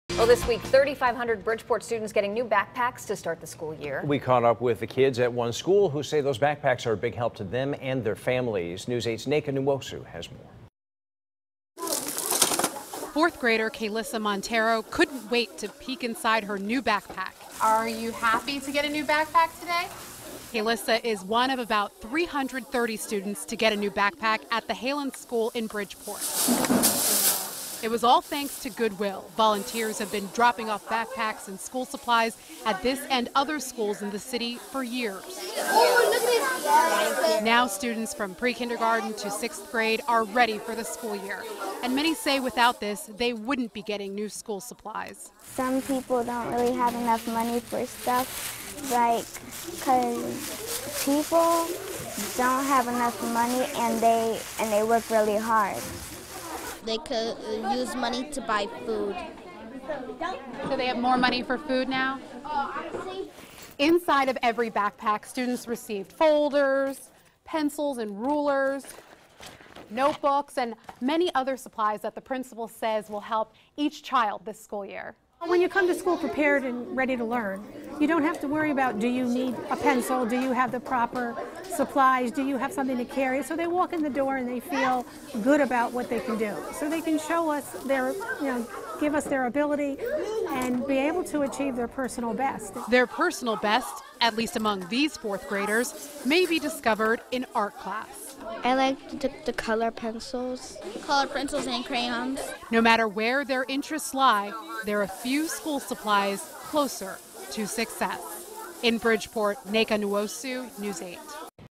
Goodwill gives Bridgeport students gifts to succeed
This week 3,500 Bridgeport students are getting new backpacks to start the school year. News 8 caught up with students at one school who say these backpacks are a big help to them and their families.